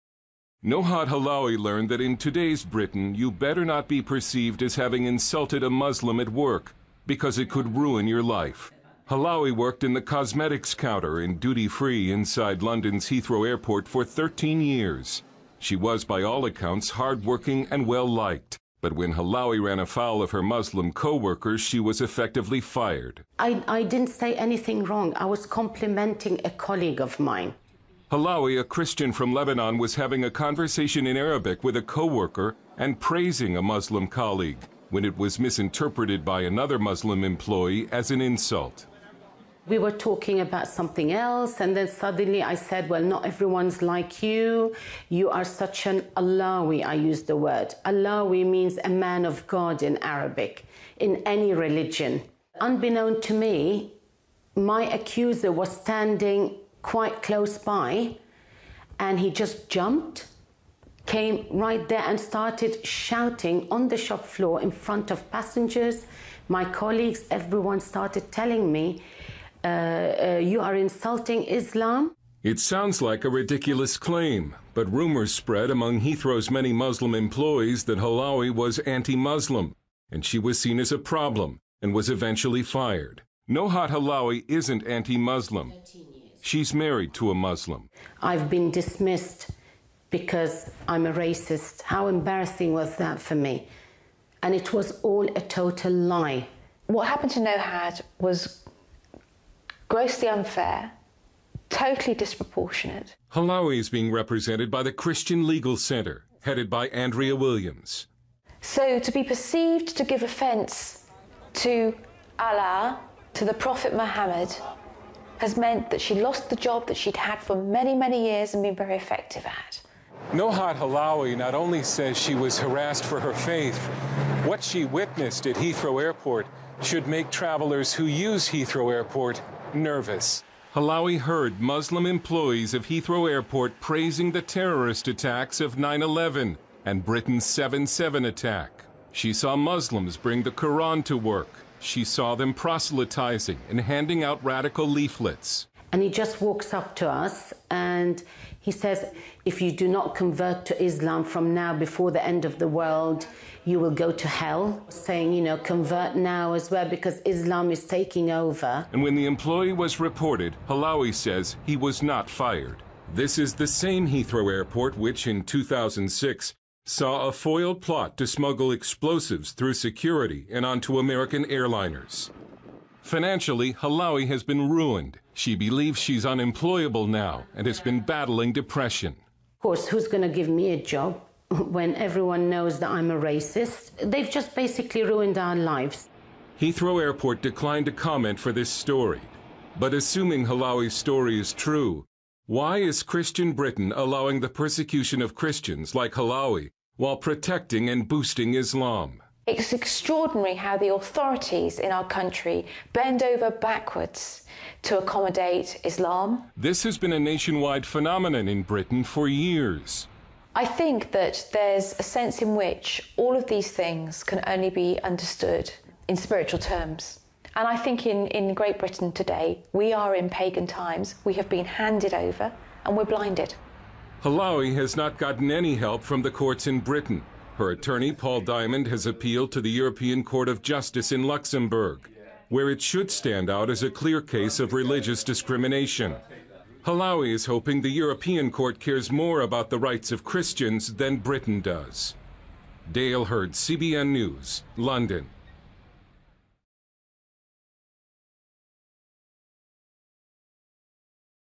L'Angleterre bientôt sous domination musulmane
L'Angleterre est un pays chrétien selon sa constitution mais la réalité montre que ce pays devient musulman. Une affaire récente de comportement anti-chrétien et pro-musulman en apporte la preuve. C'est l'histoire d'une employée chrétienne virée de l'aéroport d'Heathrow (Londres) parce qu'elle aurait soi-disant insulté un musulman...